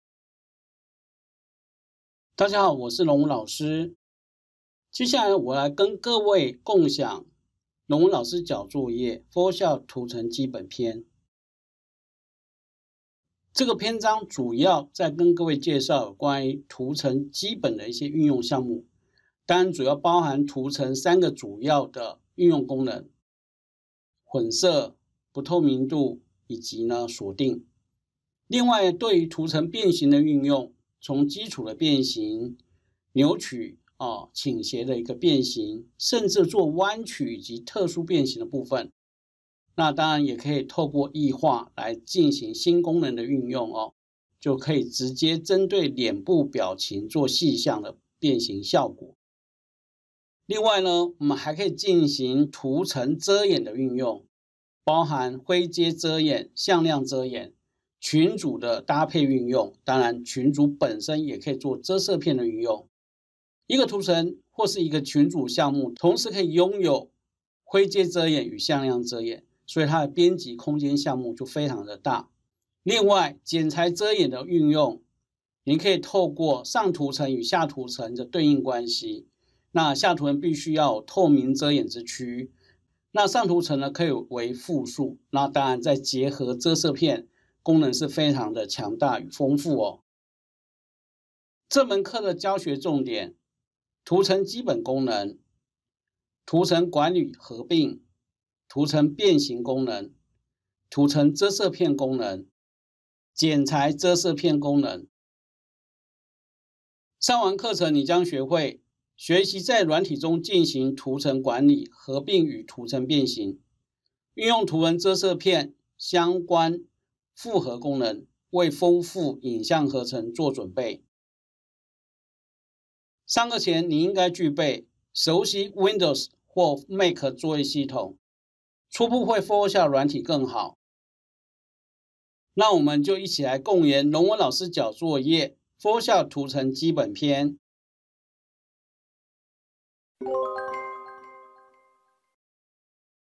龍文老師交作業-Photoshop 圖層基礎篇 黃龍文老師
Photoshop是一套專業的影像處理軟體，在攝影的世界中，更是常被使用的一套軟體，又稱其為數位暗房，深得攝影師的喜愛。Photoshop的應用範圍包括影像合成、產品造型、包裝設計、廣告宣傳、特效製作等。在設計職場領域中，適合平面設計、網頁設計、工業設計、動漫設計、遊戲製作、視訊和行動裝置等媒體設計。所以老師會錄製一系列的單完課程來幫助各位同學深化工具上的運用，不僅帶領學員了解指令操作技巧，更訓練學員思考如何運用軟體功能解決真實職場的需求與問題，本篇圖層基本篇主要幫助學員了解圖層基本運用，包含變形與圖層遮色片。